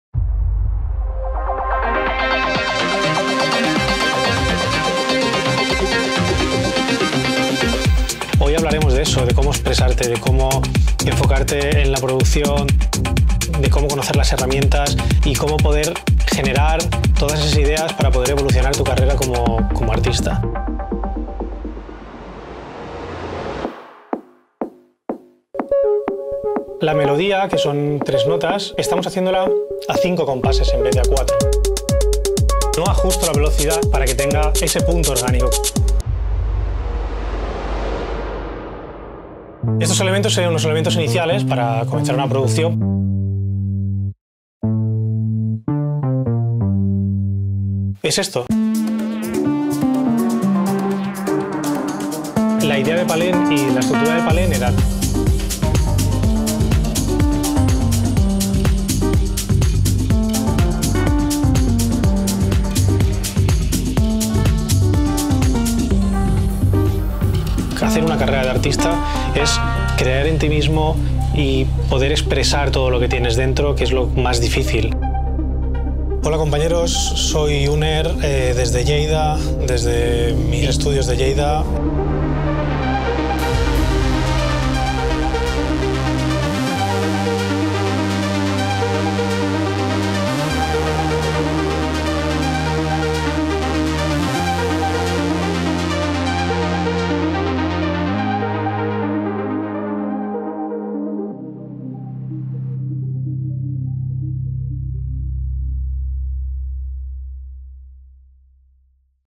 Composición y Producción de Música Electrónica | UNER.
Aprende las técnicas con las que UNER ha producido tracks para sellos como Dynamic, Cadenza, Cocoon, Toolroom o Solar Distance.

UNER es uno de los nombres más importantes de la nueva generación de DJs y productores.

Su trayectoria en la producción musical así como su figura como instructor musical, le permiten desarrollar una amplia variedad de técnicas de producción musical.

En esta Masterclass online aprenderás producción de música electrónica dentro del estudio privado de UNER, es una guía completa para descubrir como UNER utiliza Ableton Live, conocerás sus técnicas de mezcla, composición de melodías, verás como consigue que sus tracks tengan buena armonía, aprenderás arreglos de patrones, utilización de plugins, samples, estructuras de canciones, en general sabrás como desarrolla sus ideas de principio a fin.